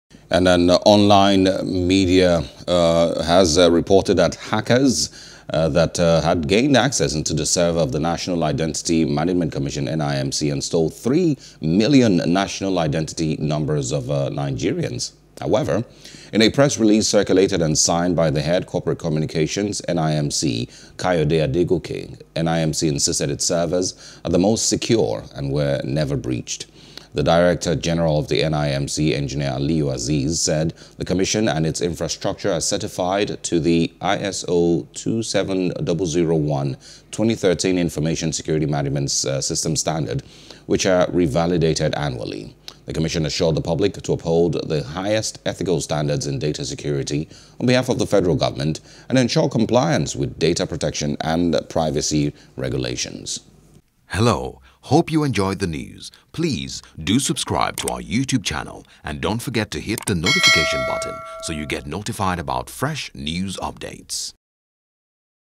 NIN Database: N.I.M.C. Denies Hacking, Says Portal Is Well Secured | NEWS
An online media had reported that hackers had gained access into the server of the National Identity Management Commission NIMC and stole three million National Identity Numbers of Nigerians.